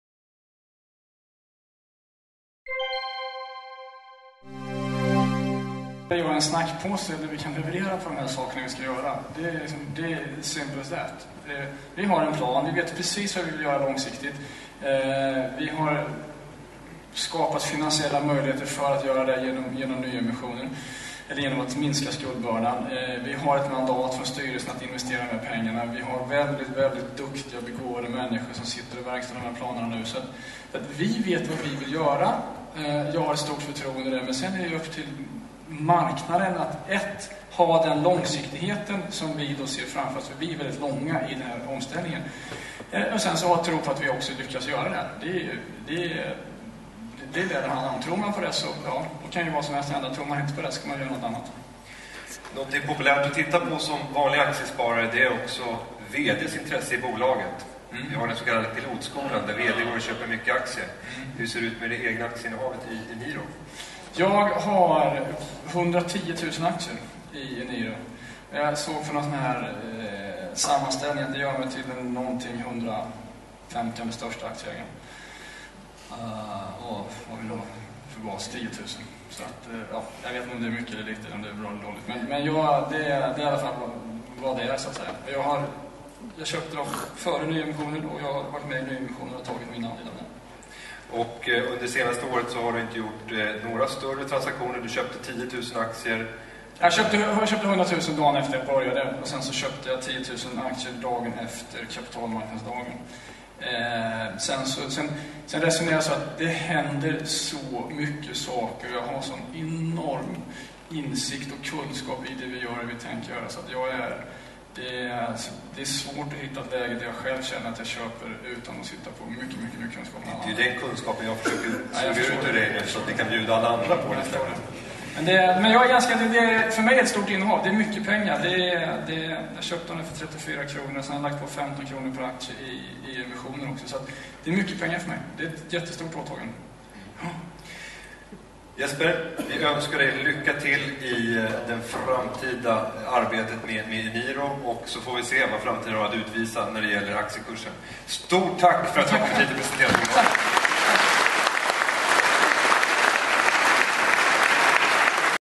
Jesper Kärrbrink på Stora Analysdagen (del 5)
Jesper Kärrbrink på Stora Analysdagen den 17/9 2009. Företagspresentation.